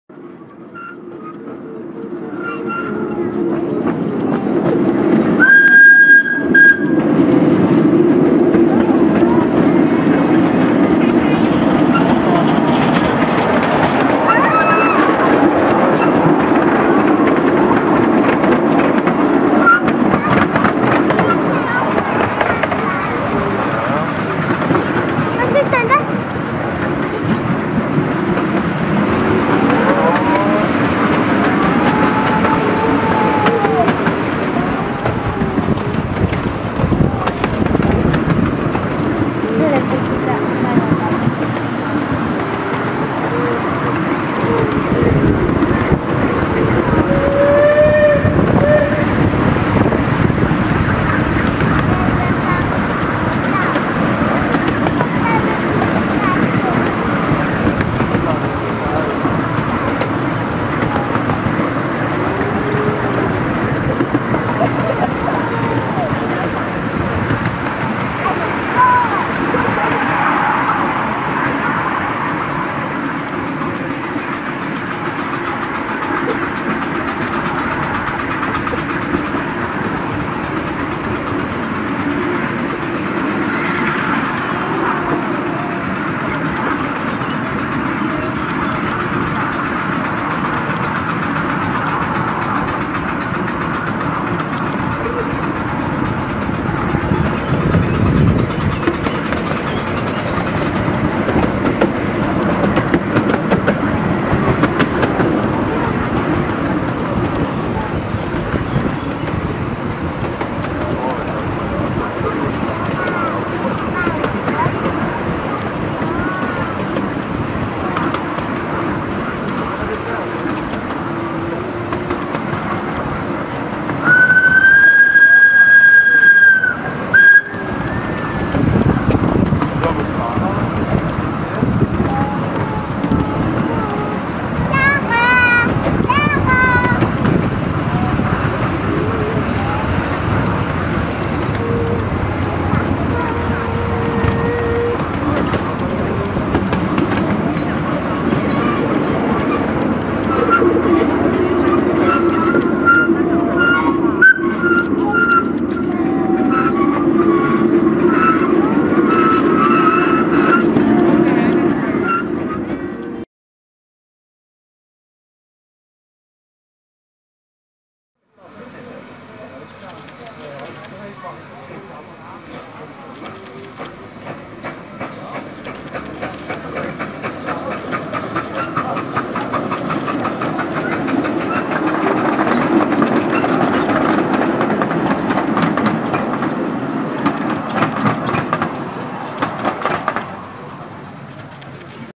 Dampfbahn-Club Auenheim Onride
Video vom Fahrtag am 01. Juni 2014 beim Dampfbahn-Club Auenheim in der Nähe von Kehl / Straßburg.